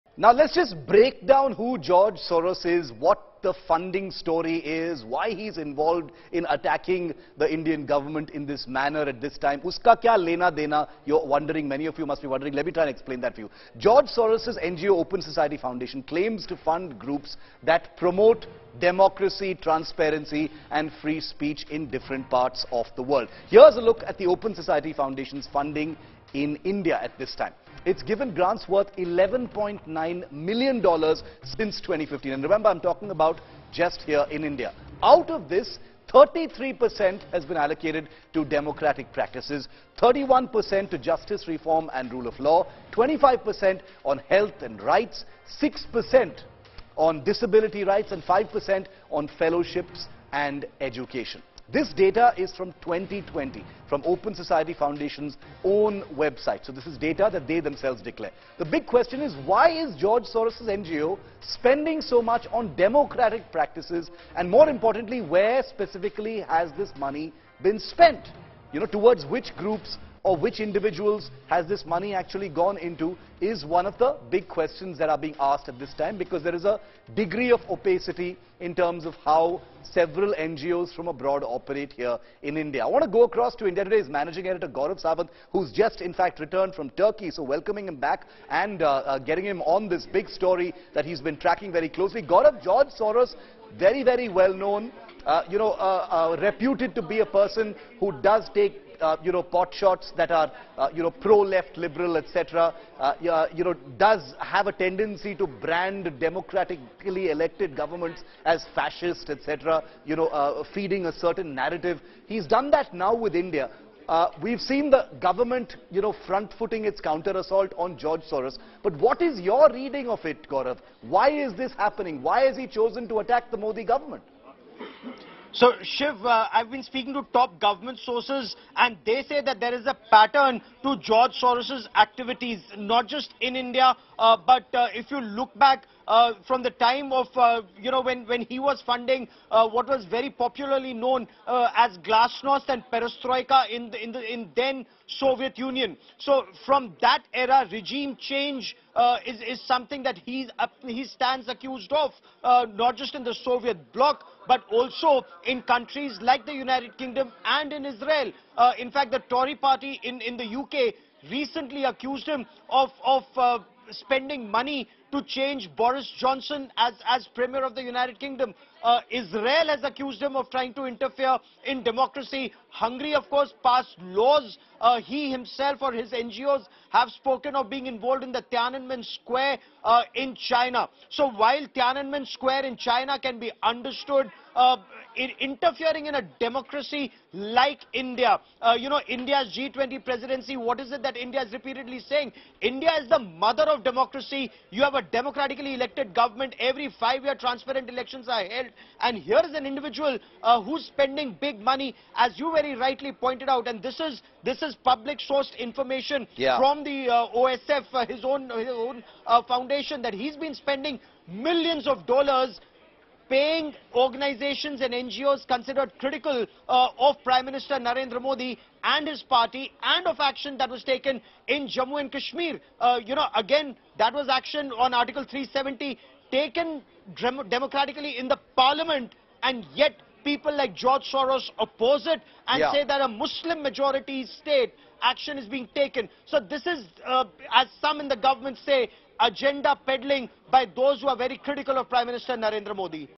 Who Is George Soros? | Why Is He Attacking Modi Government? | All You Need To Know
The Bharatiya Janata Party (BJP) on Friday launched an attack on billionaire George Soros who predicted that Prime Minister Narendra Modi will be weakened by the woes of business tycoon Gautam Adani. Union Minister Smriti Irani called upon Indians to unitedly respond to "foreign powers who try to intervene in India's democratic processes", calling his remarks "attack on India".